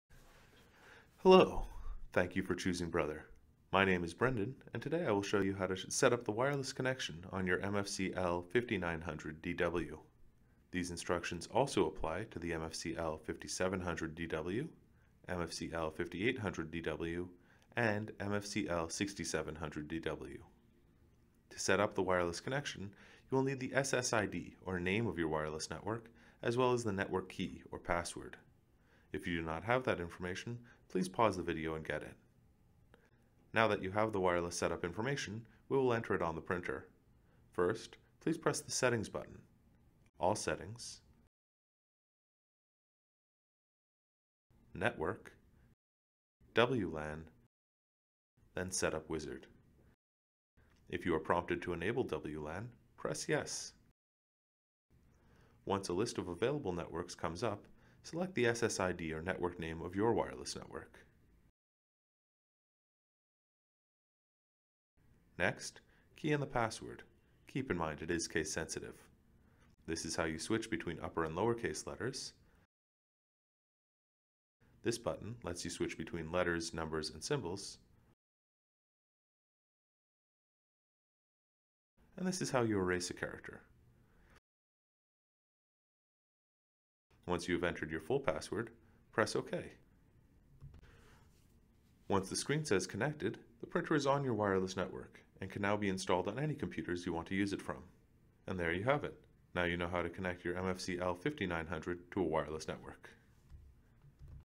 How to connect an MFCL5900DW wireless
This video shows how to use the on screen setup wizard to connect a Brother  MFCL5900DW, MFCL5700DW, MFCL5800DW or MFCL6700DW